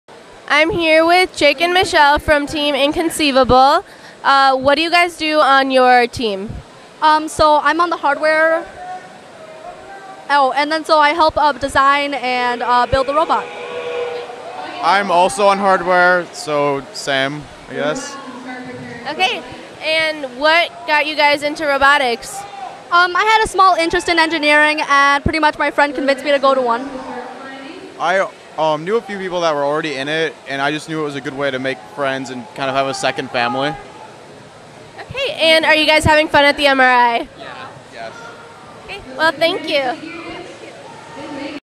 MRI Interview with team 2530 Inconceivable
Interviewing teams at the Minnesota Robotics Invitational